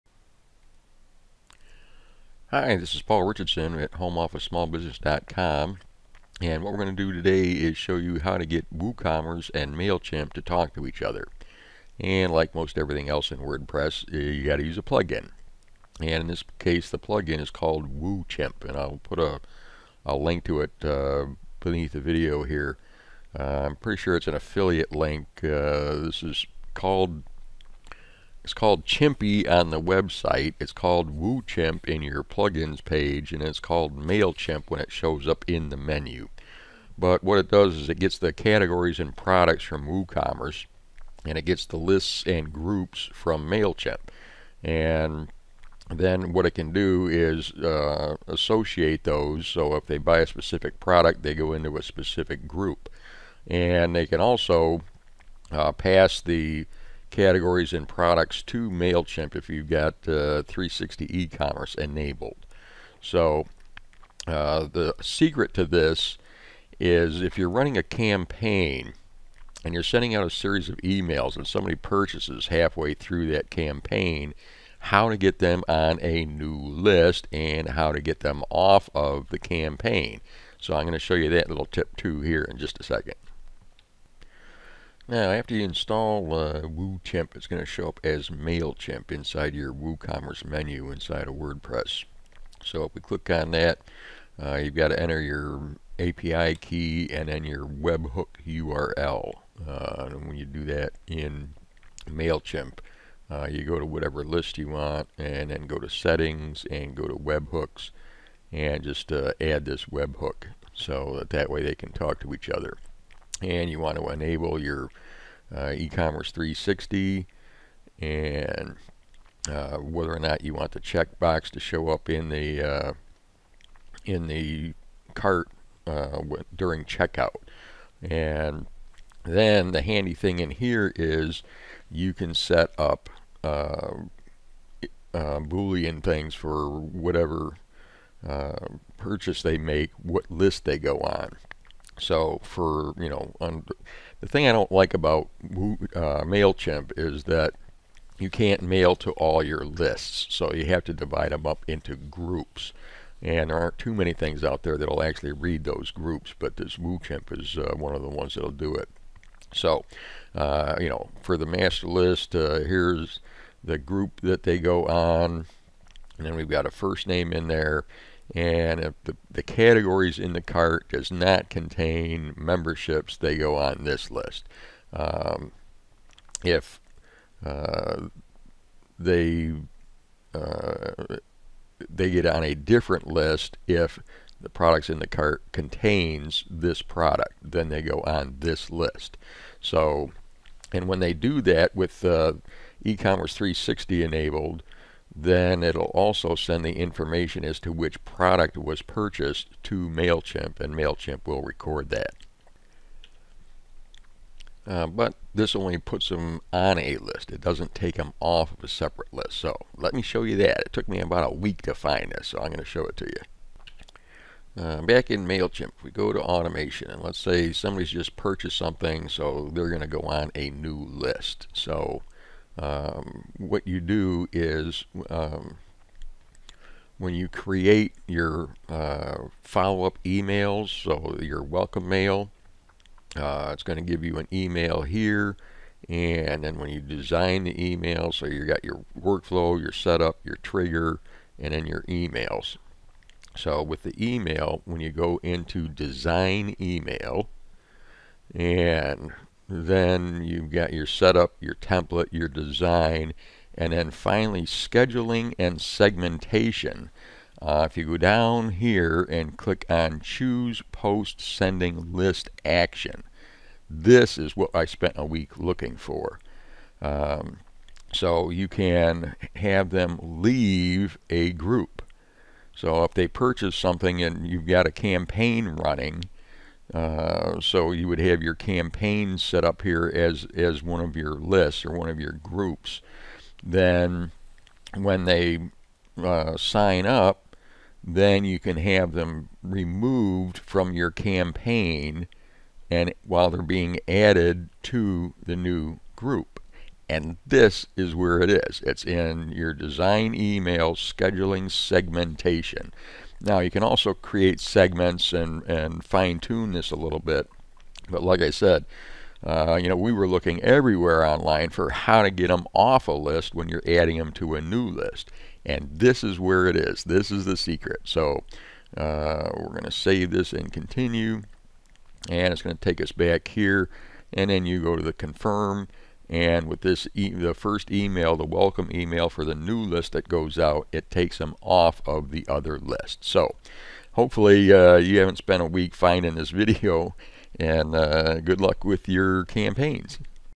How To Connect WooCommerce and MailChimp
A plugin that allows WooCommerce purchasers to be added to MailChimp Lists or Groups and how to remove new contacts from another list or group in MailChimp